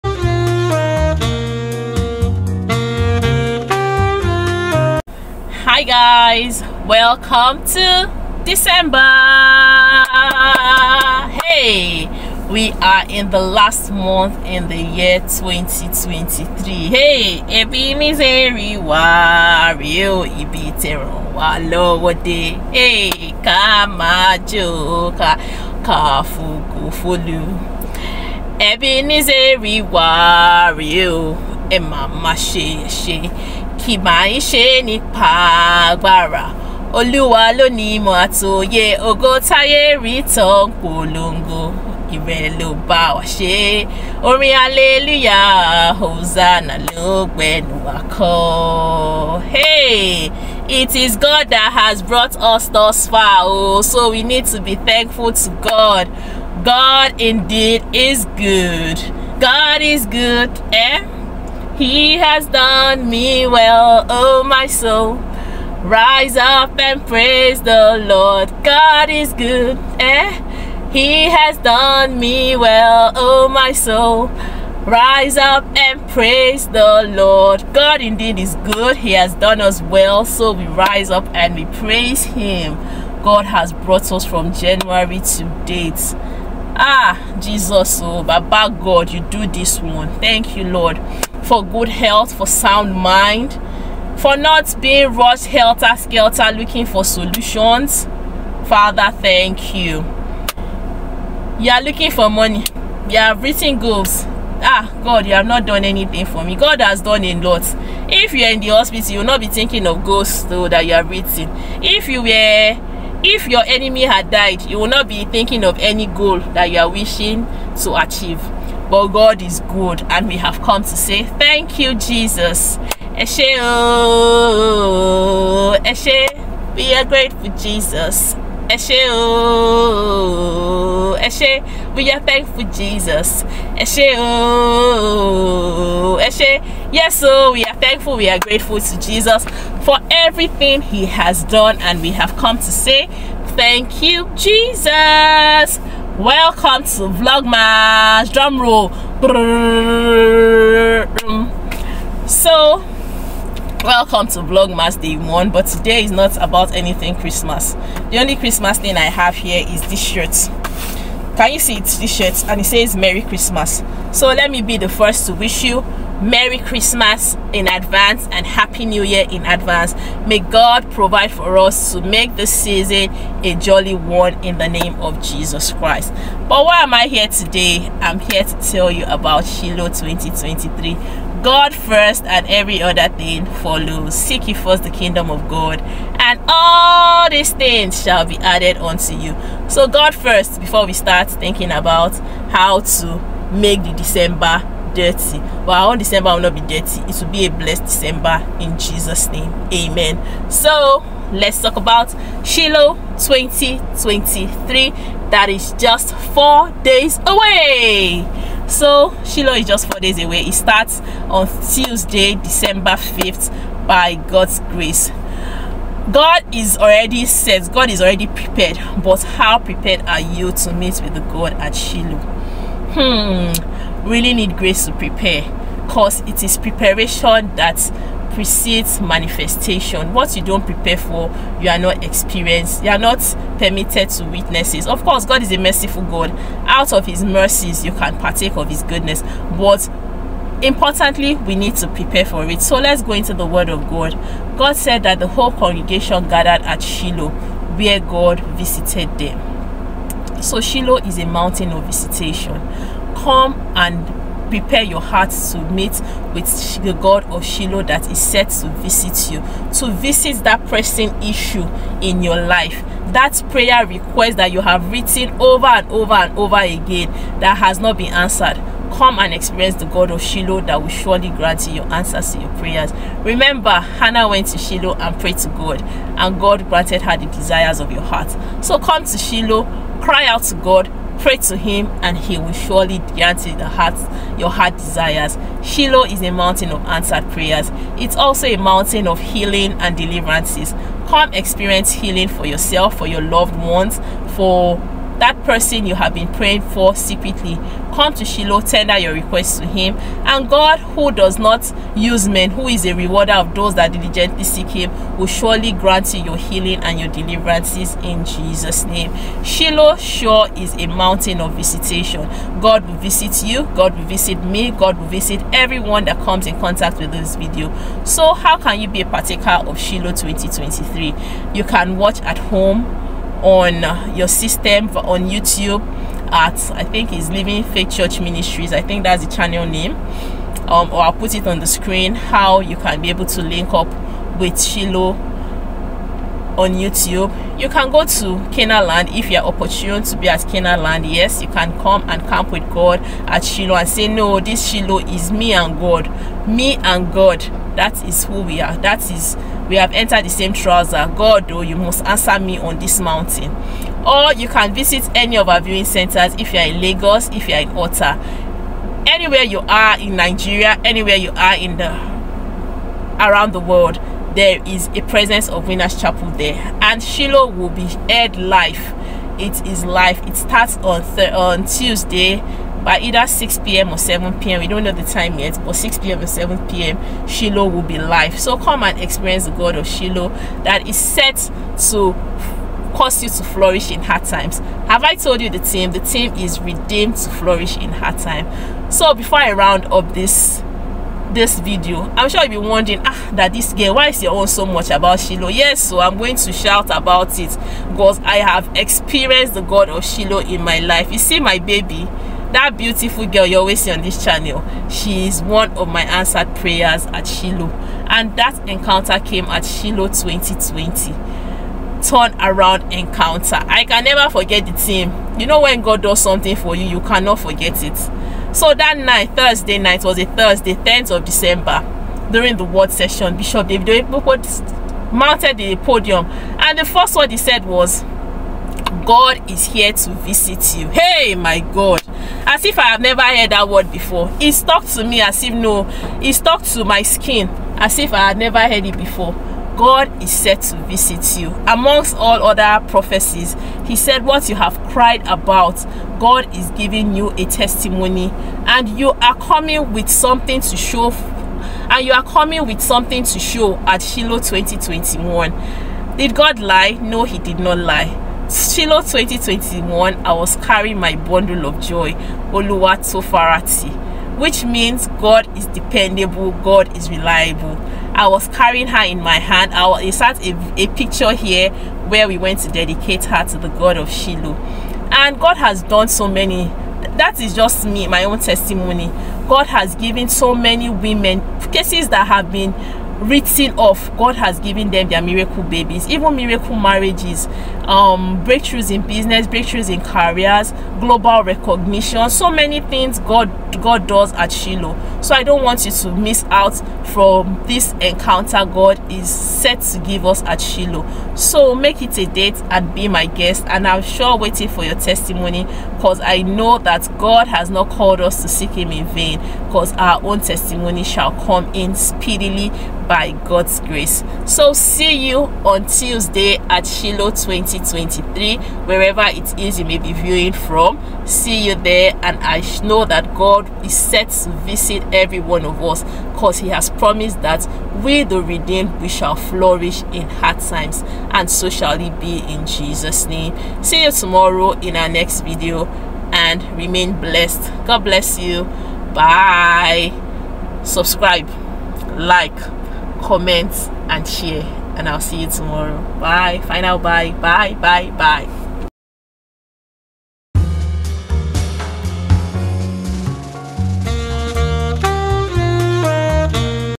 WHY YOU SHOULD ATTEND SHILOH 2023 - VLOGMAS 1
Welcome to December everyone and welcome to Vlogmas.

The long awaited Shiloh 2023 is here. How prepared are you for this great visitation.

In this video, I spoke about the whole essence of Shiloh and what to expect from this event. God surely will visit us and grant us all our heart desires.

Will you be attending?

God bless you all and may your Christmas indeed be a memorable one.

Josh18v1 - And the whole congregation of the children of Israel assembled together at Shiloh, and set up the tabernacle of the congregation there. And the land was subdued before them.

IG and TikTok - @sisiladsapce